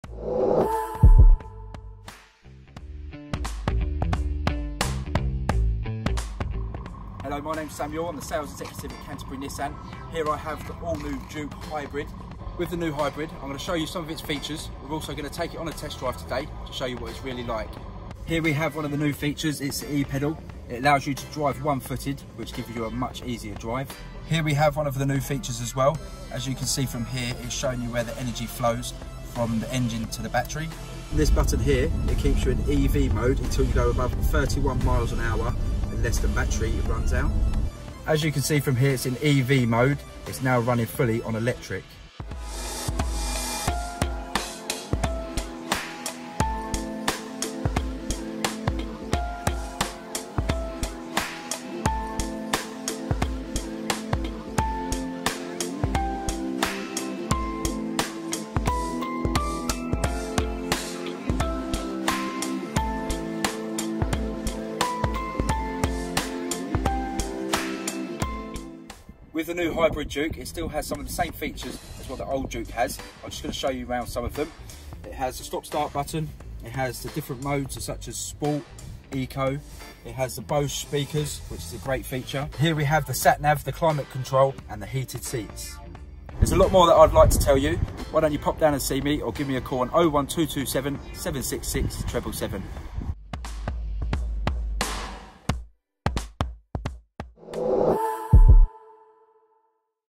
Nissan Juke Hybrid now available at Canterbury Nissan.
The Nissan Juke with upgraded Hybrid drive, now available at our Canterbury dealership.